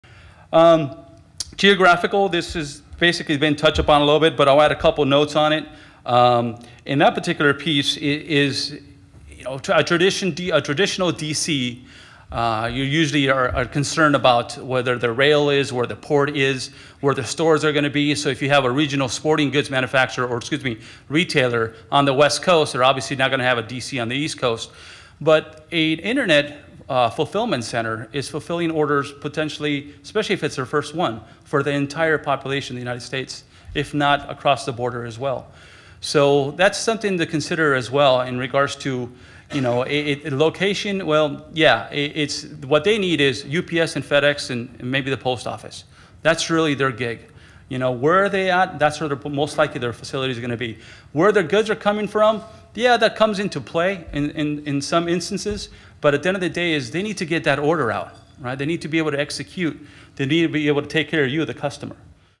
What's Next for Internet Fulfillment Centers
Speaker: Carlos Vega, regional director-west coast sales, Dematic

The rapidly expanding e-commerce sector has resulted in a shift in site selection and building requirements to meet the unique and specific needs of this industry. Traditional, brick-and-mortar retailers are expanding into the e-commerce market to meet consumer demands that are running at 17 percent growth year over year. This transition by the traditional retailers to support both storefront and e-commerce consumers has resulted in a few different models that will be discussed during this session.

Our panel includes a developer who has completed a large e-commerce fulfillment center and an expansion, an industry leader in fulfillment center equipment who will address some of the new building specs that are required, and a COO from a burgeoning e-fulfillment operation with an inside look at his facility. The session will also cover the current and future impact of e-commerce on geographical site requirements, along with the opportunities and challenges that the traditional retailers are facing as they modify their supply chains, stores and distribution network to support their customers in store and online purchases.